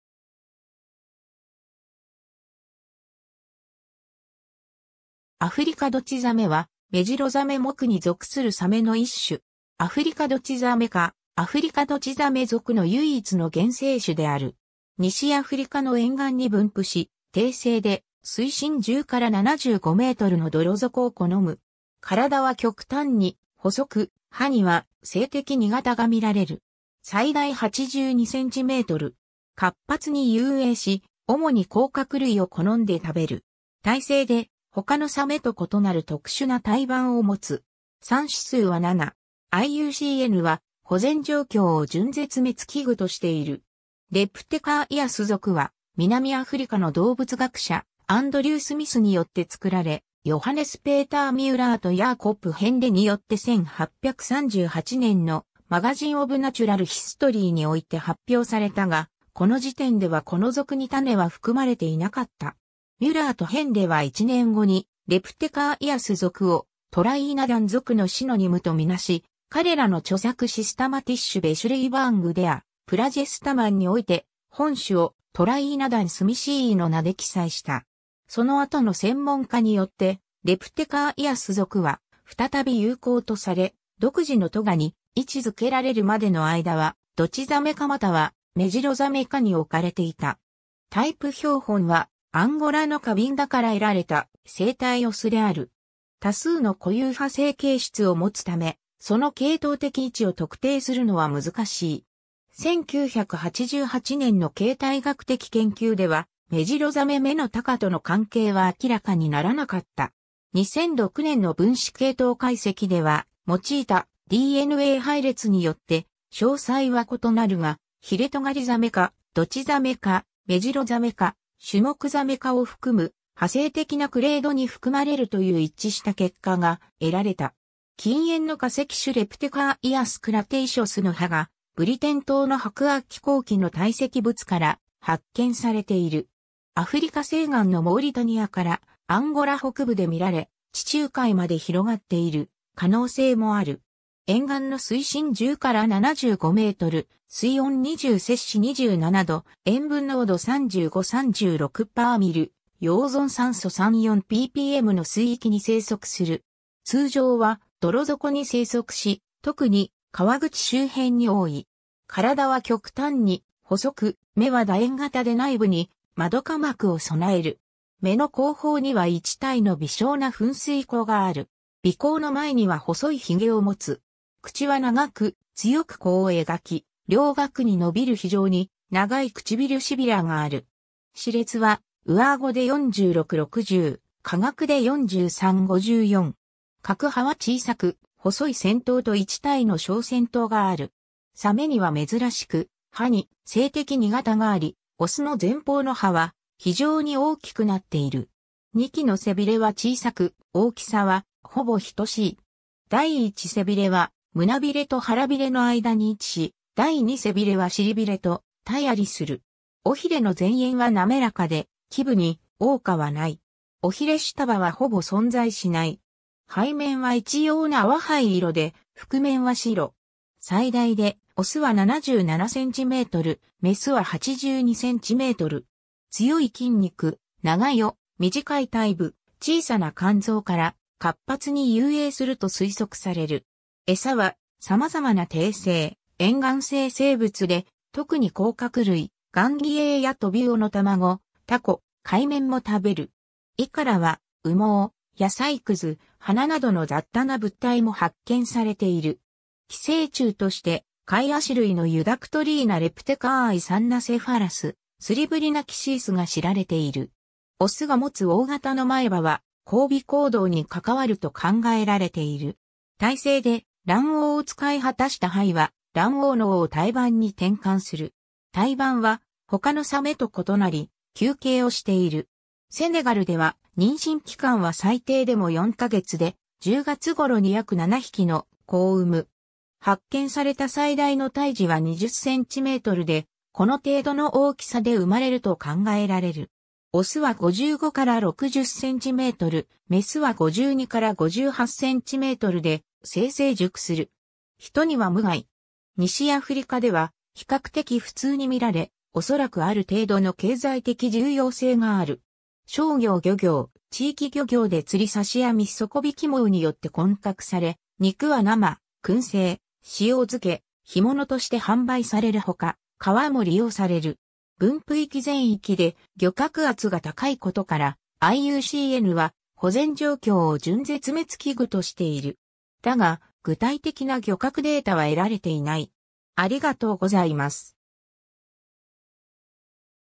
アフリカドチザメ
アフリカドチザメ (Leptocharias smithii) はメジロザメ目に属するサメの一種。
アフリカドチザメ科・アフリカドチザメ属の唯一の現生種である。
西アフリカの沿岸に分布し、底生で水深10-75mの泥底を好む。
体は極端に細く、歯には性的二型が見られる。
最大82cm。
 活発に遊泳し、主に甲殻類を好んで食べる。
胎生で、他のサメと異なる特殊な胎盤を持つ。
産仔数は7。
IUCNは保全状況を準絶滅危惧としている。
 Leptocharias 属は南アフリカの動物学者アンドリュー・スミスによって作られ、ヨハネス・ペーター・ミュラーとヤーコプ・ヘンレによって1838年のMagazine of Natural History において発表されたが、この時点ではこの属に種は含まれていなかった。
ミュラーとヘンレは1年後にLeptocharias 属をTriaenodon 属（ネムリブカ属）のシノニムとみなし、彼らの著作Systematische Beschreibung der Plagiostomen において本種をTriaenodon smithii の名で記載した。
その後の専門家によってLeptocharias 属は再び有効とされ、独自の科に位置づけられるまでの間はドチザメ科またはメジロザメ科に置かれていた。
タイプ標本はアンゴラのカビンダから得られた成体雄である。
 多数の固有派生形質を持つため、その系統的位置を特定するのは難しい。
1988年の形態学的研究では、メジロザメ目の他科との関係は明らかにならなかった。
2006年の分子系統解析では、用いたDNA配列によって詳細は異なるが、ヒレトガリザメ科・ドチザメ科・メジロザメ科・シュモクザメ科を含む派生的なクレードに含まれるという一致した結果が得られた。
近縁の化石種Leptocharias cretaceus の歯が、ブリテン島の白亜紀後期（サントニアン-カンパニアン、8600–7200万年前）の堆積物から発見されている。
 アフリカ西岸のモーリタニアからアンゴラ北部で見られ、地中海まで広がっている可能性もある。
沿岸の水深10-75m、水温20-27℃、塩分濃度35–36‰、溶存酸素3–4 ppmの水域に生息する。
通常は泥底に生息し、特に河口周辺に多い。
 体は極端に細く、眼は楕円形で内部に瞬膜を備える。
眼の後方には1対の微小な噴水孔がある。
鼻孔の前には細い髭を持つ。
口は長く、強く弧を描き、両顎に伸びる非常に長い唇褶がある。
歯列は上顎で46–60・下顎で43–54。
各歯は小さく、細い尖頭と1対の小尖頭がある。
サメには珍しく、歯に性的二型があり、雄の前方の歯は非常に大きくなっている。
2基の背鰭は小さく、大きさはほぼ等しい。
第一背鰭は胸鰭と腹鰭の間に位置し、第二背鰭は臀鰭と対在する。
尾鰭の前縁は滑らかで基部に凹窩はない。
尾鰭下葉はほぼ存在しない。
背面は一様な淡灰色で腹面は白。
最大で雄は77cm・雌は82cm。
 強い筋肉、長い尾、短い体部、小さな肝臓から、活発に遊泳すると推測される。
餌は様々な底生・沿岸性生物で、特に甲殻類（カニ・ロブスター・エビなど) を好むが、小さな硬骨魚（イワシ・アンチョビ・ウミヘビ科・ギンポ亜目・ハゼ・カレイ目など）・ガンギエイやトビウオの卵・タコ・海綿も食べる。
胃からは羽毛・野菜屑・花などの雑多な物体も発見されている。
寄生虫としてカイアシ類のEudactylina leptochariae ・Thamnocephalus cerebrinoxius が知られている。
 雄が持つ大型の前歯は、交尾行動に関わると考えられている。
胎生で、卵黄を使い果たした胚は卵黄嚢を胎盤に転換する。
胎盤は他のサメと異なり、球形をしている。
セネガルでは妊娠期間は最低でも4ヶ月で、10月頃に約7匹の仔を産む。
発見された最大の胎児は20cmで、この程度の大きさで産まれると考えられる。
雄は55-60cm・雌は52-58cmで性成熟する。
 人には無害。
西アフリカでは比較的普通に見られ、おそらくある程度の経済的重要性がある。
商業漁業・地域漁業で釣り・刺し網・底引き網によって混獲され、肉は生・燻製・塩漬け・干物として販売されるほか、革も利用される。
分布域全域で漁獲圧が高いことから、IUCNは保全状況を準絶滅危惧としている。
だが具体的な漁獲データは得られていない。